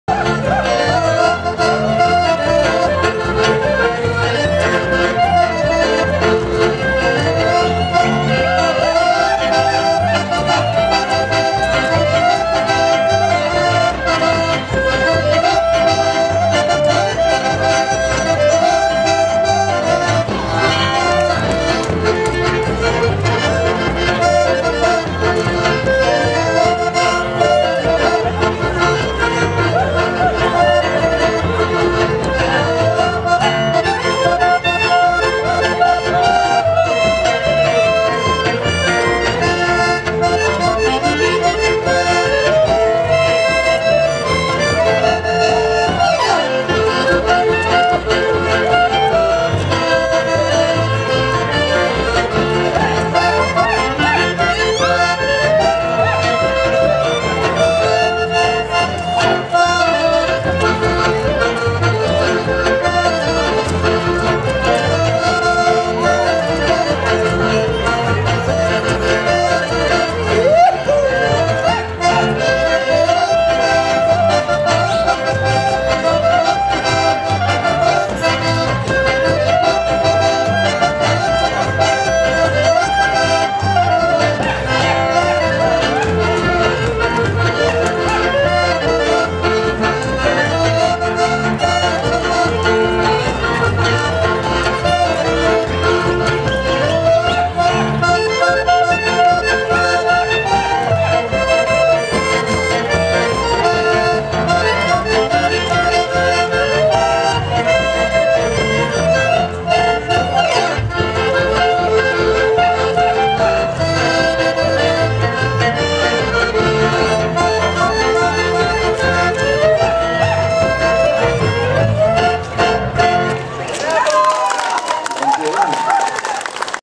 MOV01B
XX Lubuski Przegląd Zespołów i Grup Śpiewaczych "Pejzaż muzyką malowany". Gościeszowice, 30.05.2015 r., plac przy Gminnym Centrum Kultury.